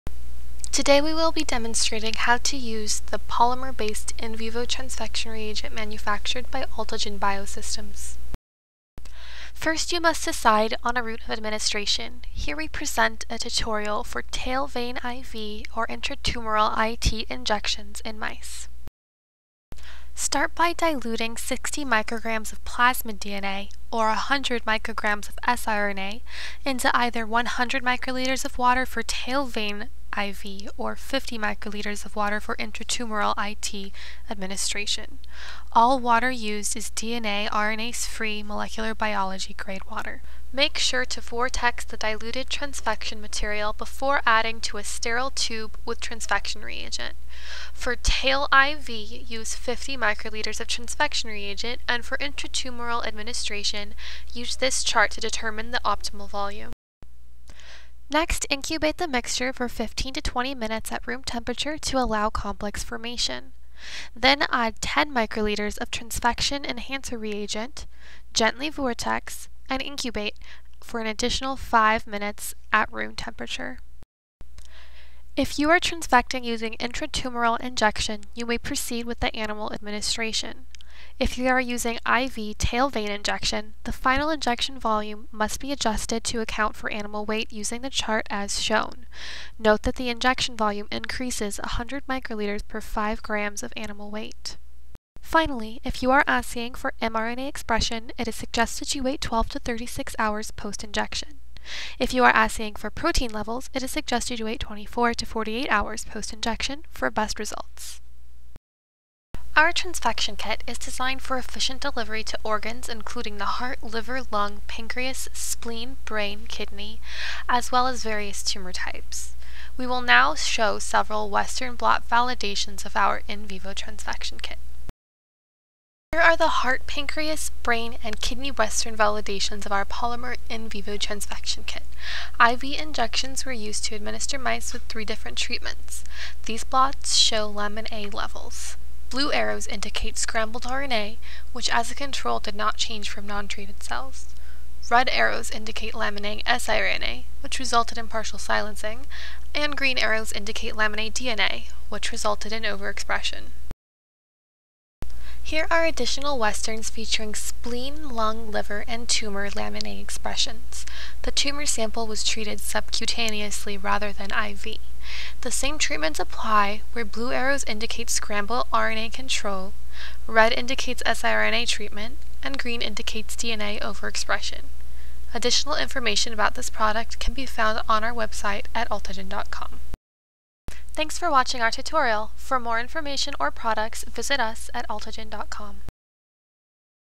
Altogen Biosystems Polymer in vivo Transfection Tutorial
Cationic polymers such as polyethylenimine and DEAE-dextran are used to form complexes with nucleic acid and transfect cells via endocytosis. Some polymers have been developed to form micelles; Altogen Biosystems developed a proprietary biodegradable polymer-based transfection reagent for in vivo use that results in efficient and minimally toxic delivery of DNA/RNA material to the lung, liver, kidney and spleen via a single i.v. injection that results in sustainable knockdown or overexpressions. This product has been tested in mice using systemic intravenous as well as direct intratumoral injections. More information about this product can be found at: